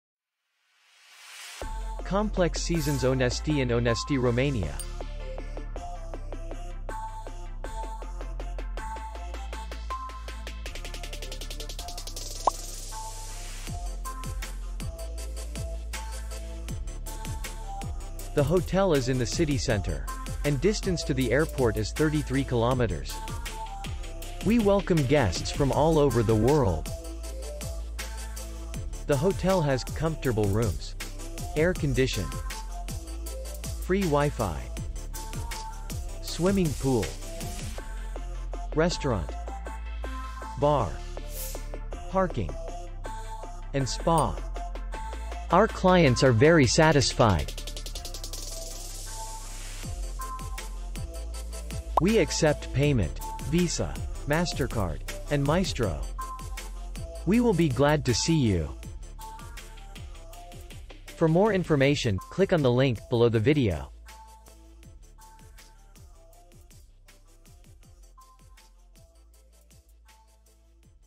Complex Seasons Onesti hotel review | Hotels in Onesti | Romanian Hotels
Complex Seasons Onesti hotel video review. Hotels in Onesti, european 3 stars hotels.